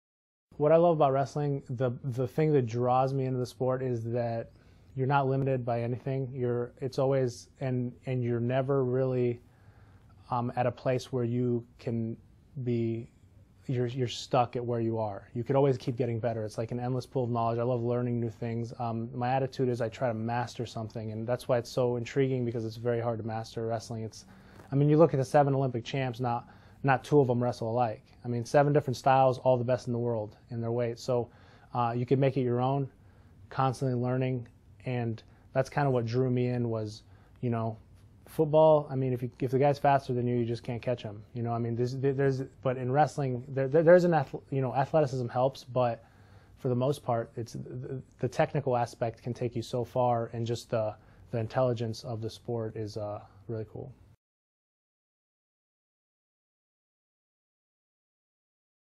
Tervel Dlagnev | Constantly Learning
2012 Olympic wrestler Tervel Dlagnev shares why he loves the sport of wrestling. "What I love about wrestling, the thing that drives me in the sport is that you are not limited by anything and you are never really at a place where you are stuck at where you are. You can always get better."